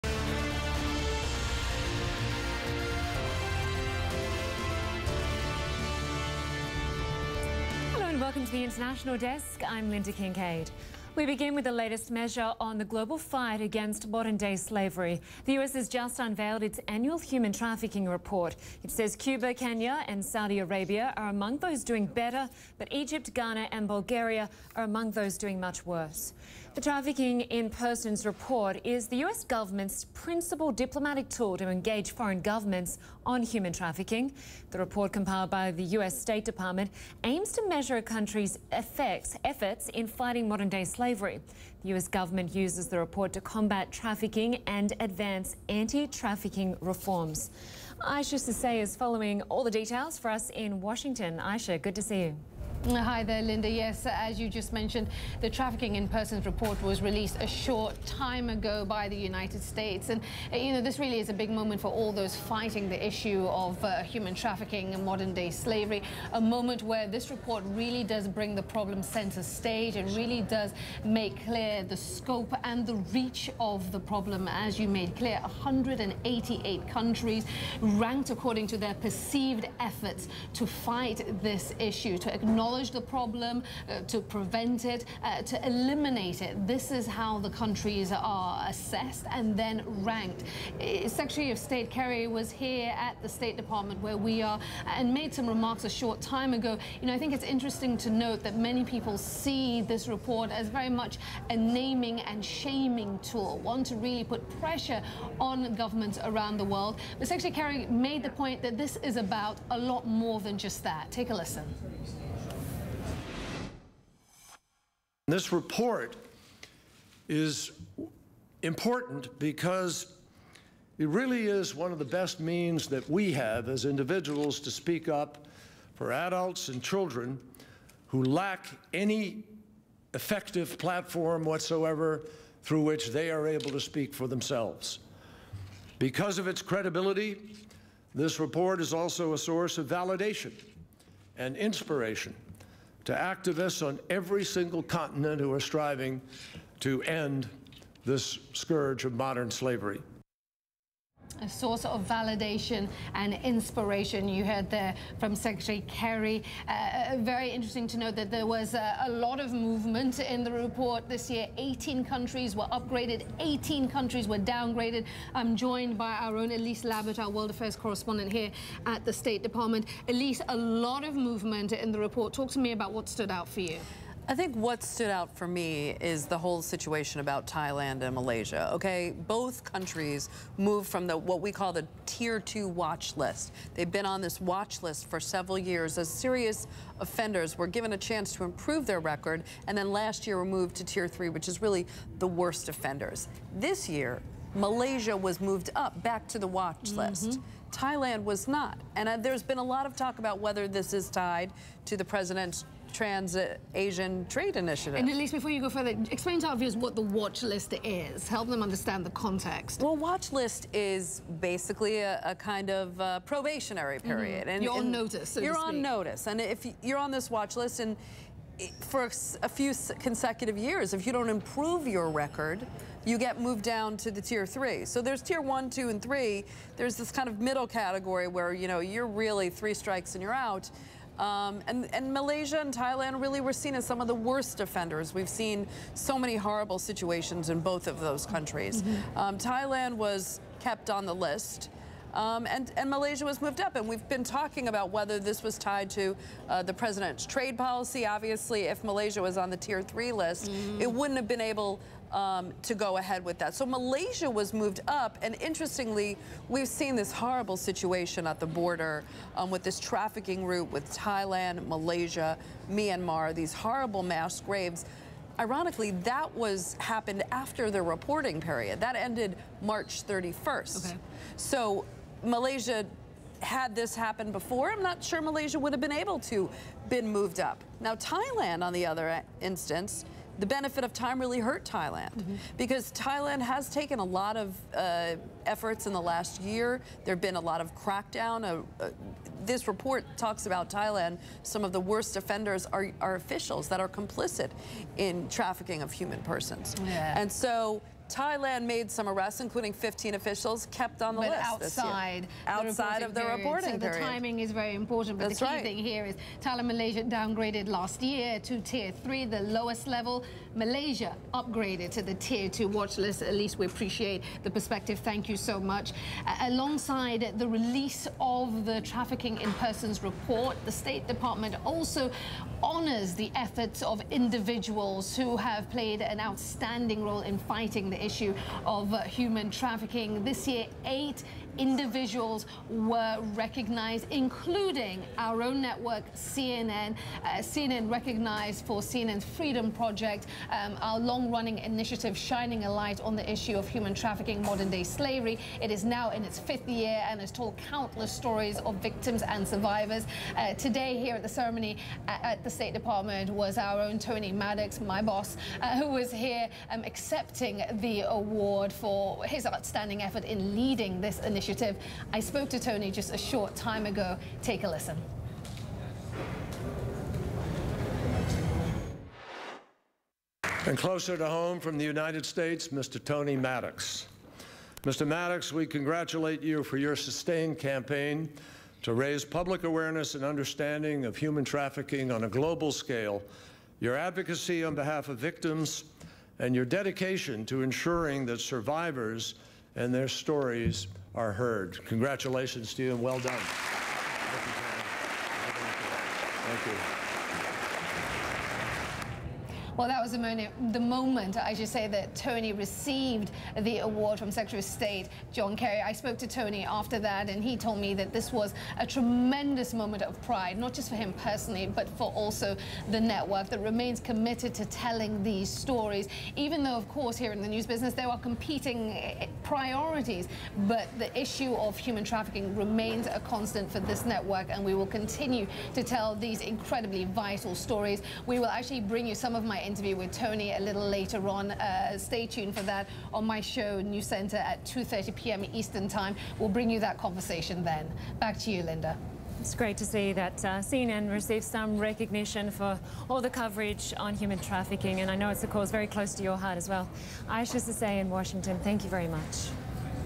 Human trafficking report: which countries improved and which fell behind?
CNN's Isha Sesay has the latest from Washington DC on the 2015 Trafficking in Persons (TIP) report which assesses and rates around 200 countries on their response to human trafficking, forced labour and sex exploitation.
CNN International anchored by Lynda Kinkade.